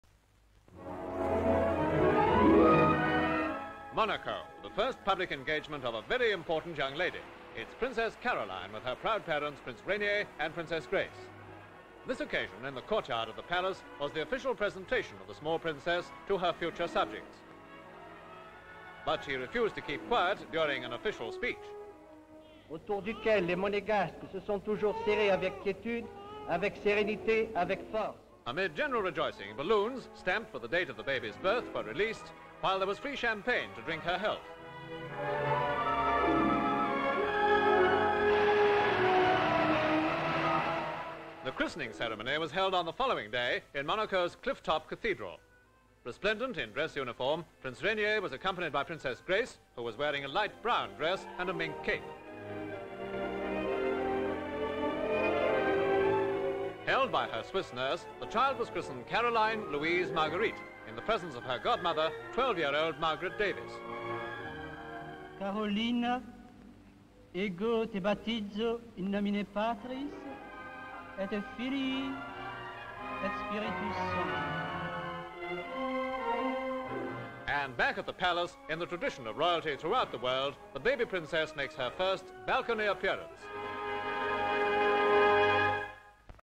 MONACO: CHRISTENING OF PRINCESS CAROLINE: (1957)
BALLOONS. with the date of birth of Princess Caroline on them are released from the Palace courtyard in Monaco

CAROLINE, Princess. The Christening of. Grace holding the baby, with Rainier speaking into microphone (silent) in the courtyard of the Palace. During an official speech she makes a noise. Balloons with her birth date printed on them are released into the air. Palace in the background. Grace & Rainier toast her health in champagne in courtyard. At the Cathedral she is christened..held by her nurse (swiss) in presence of her Godmother..12 yr old Margaret Davis. All three make their appearance on the balcony... first public appearance on the balcony for

PRINCESS GRACE OF MONACO (formerly Grace Kelly of Hollywood) The Christening of Princess Caroline...Grace holding the baby in her arms in the courtyard. Standing near Rainier who is talking into Mike. Going into the cathedral for the Christening...walking down the aisle. on the balcony on the palace with her husband and the baby

MONACO The Christening of Princess Caroline (full details see her card I) The Courtyard of the Palace..The palace when baloons are released...VG shot of the Cathedral

RAINIER. PRINCE OF MONACO The Christening of Princess Caroline. Rainier speaking into microphone (silent) Standing by his wife. Grace and...toast the baby's health...In State dress he and his wife go up the steps to the Cathedral. Inside. On the balcony of the palace...all 3 wave to the crowds

RELIGIOUS: (PROCESSIONS: CEREMONIES SERVICES) The Christening of Princess Caroline in Monacos Cathedral...clery standing around etc

Monaco; Rites of Passage; Royalty
Background: CHRISTENING OF PRINCESS CAROLINE: